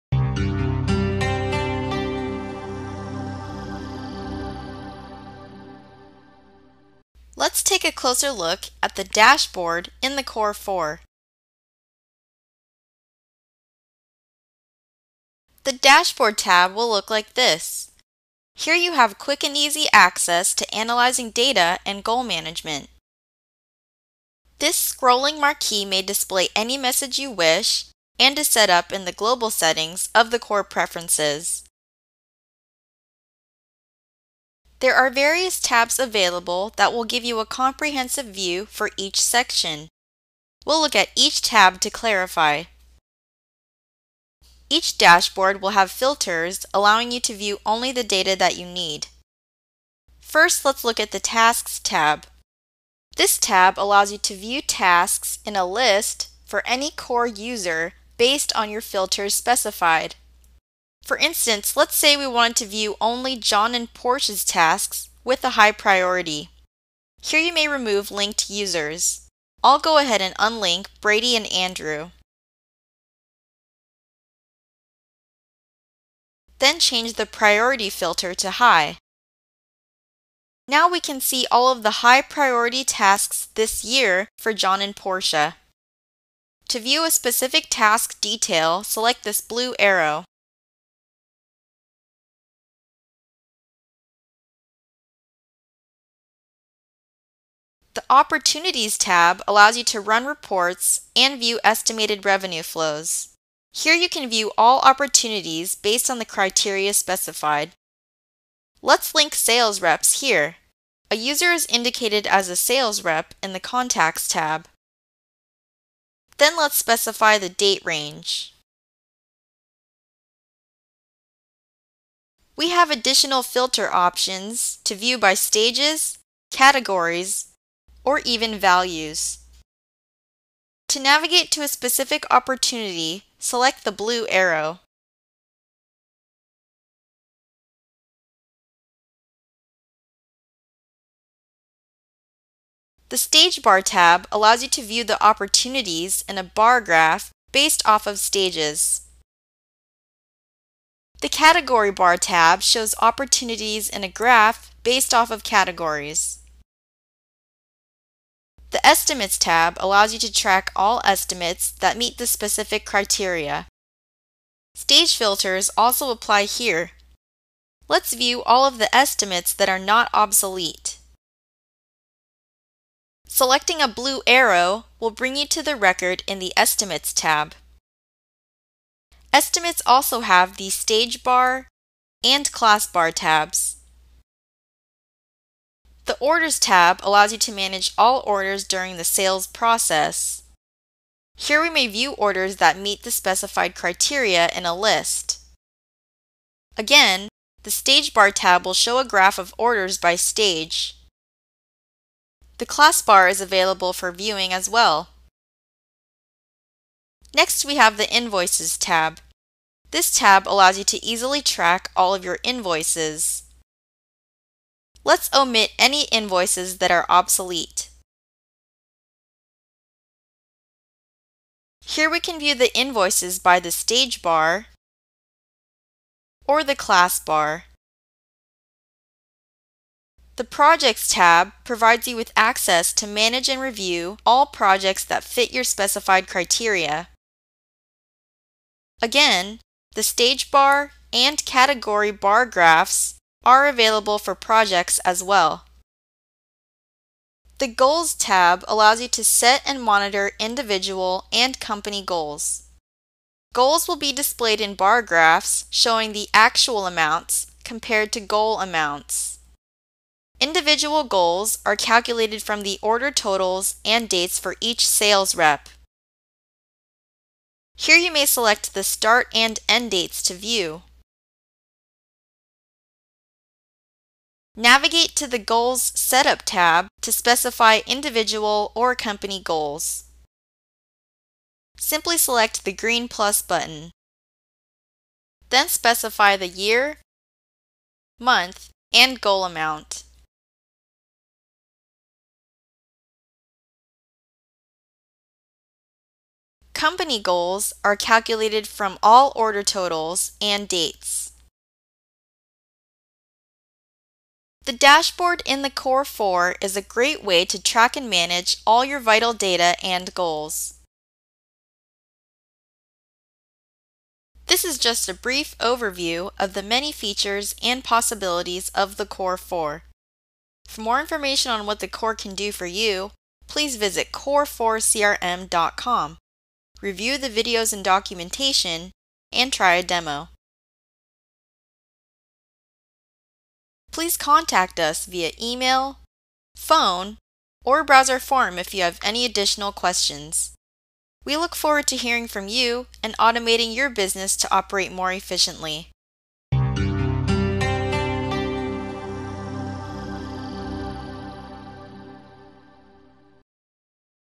18. Dashboard - Core4 CRM
This video provides an overview of the Dashboard in the Core4 CRM.

to try a product demo, read the product documentation, or view all tutorial videos.